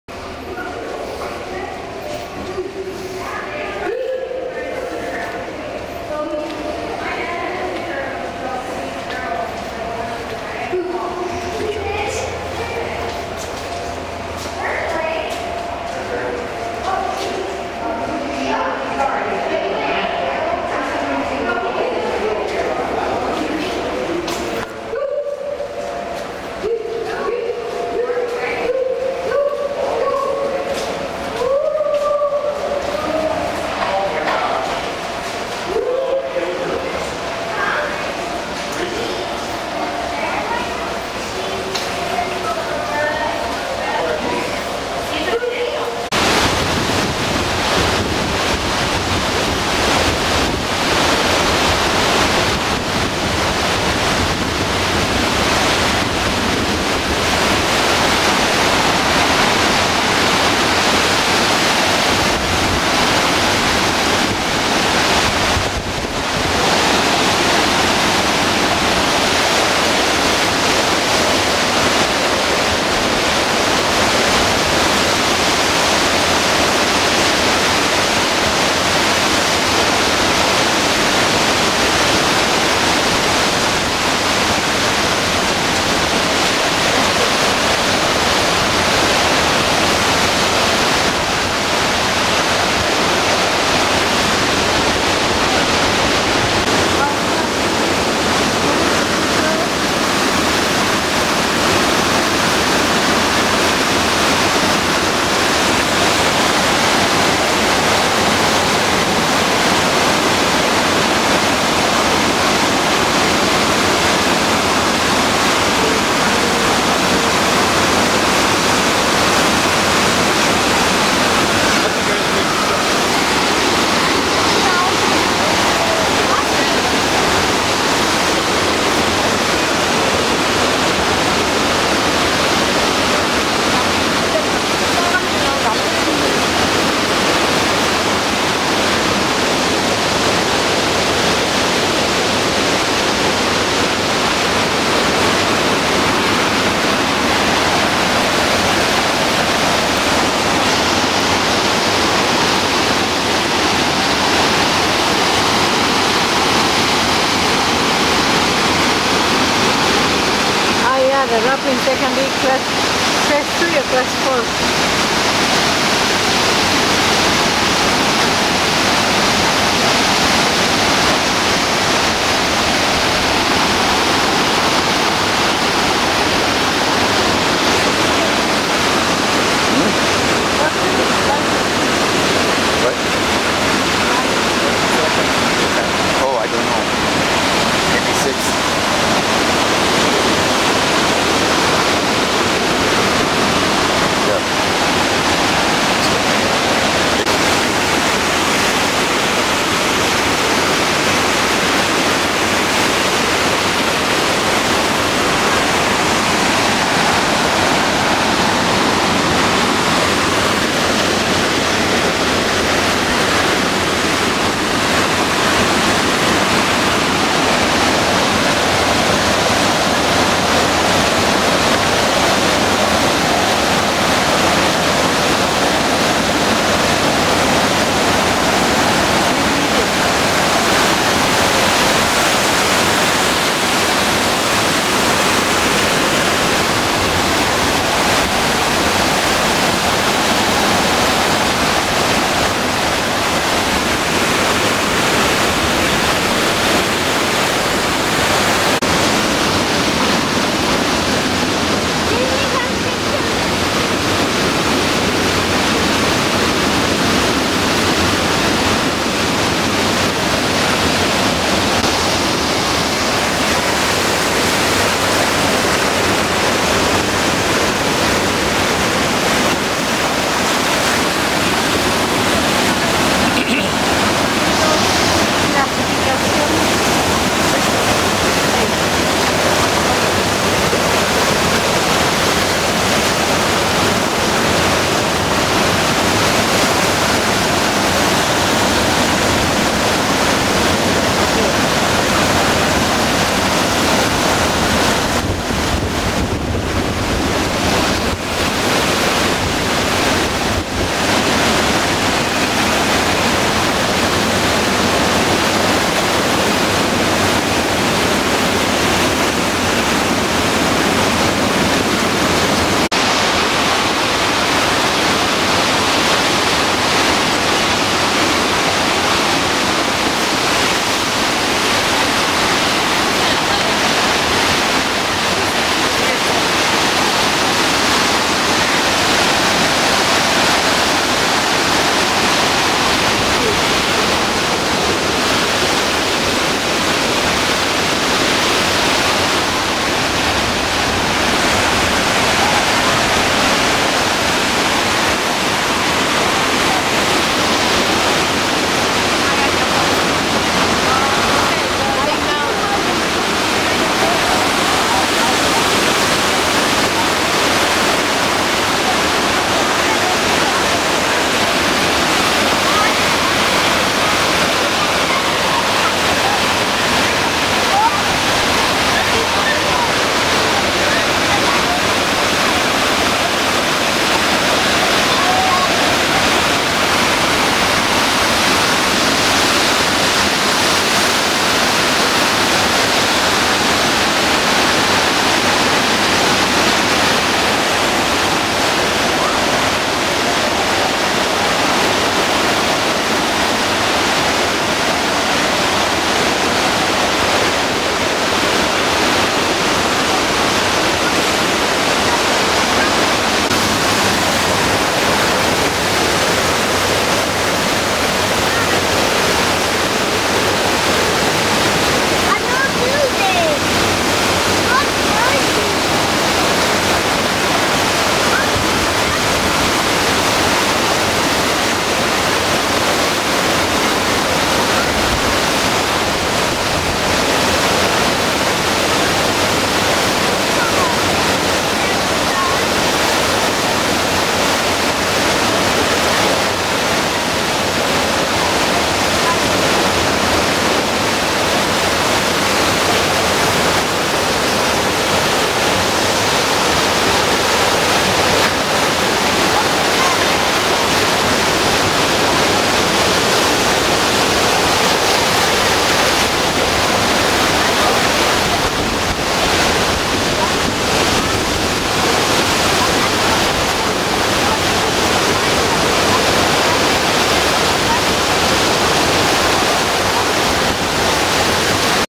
Niagara Falls white water rapids walk. June 2016
Took an old elevator down 200 feet at Niagara Falls to walk at the white water rapids...